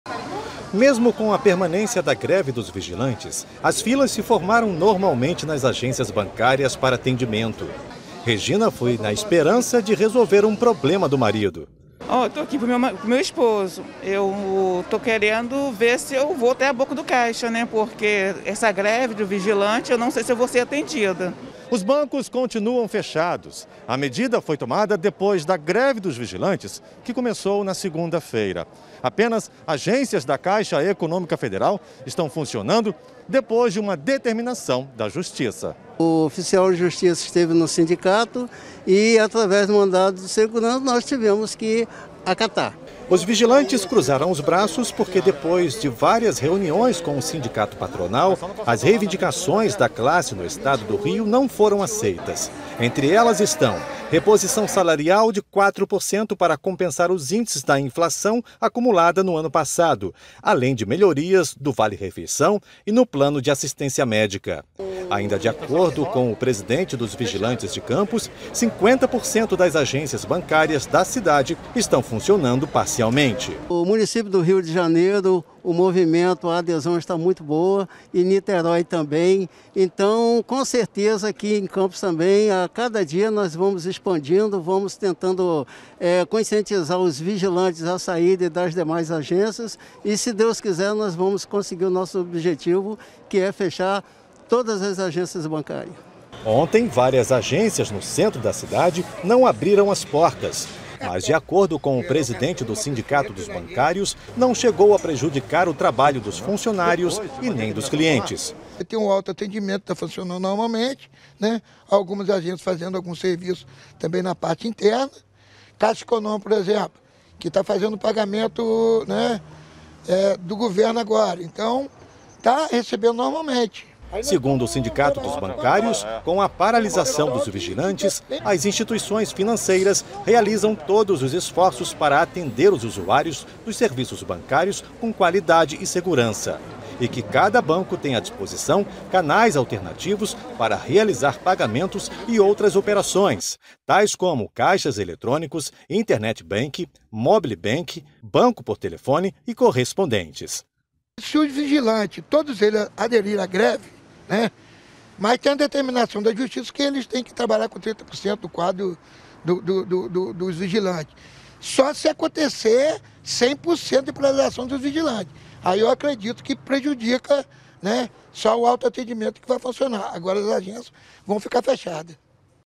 Vigilantes entram no terceiro dia de greve
Cerca de 50% das agências bancárias de Campos estão funcionando parcialmente. Isso porque, há três dias, vigilantes cruzaram os braços por não terem chegado a um consenso sobre reivindicações trabalhistas. Canais alternativos têm sido a saída para os usuários.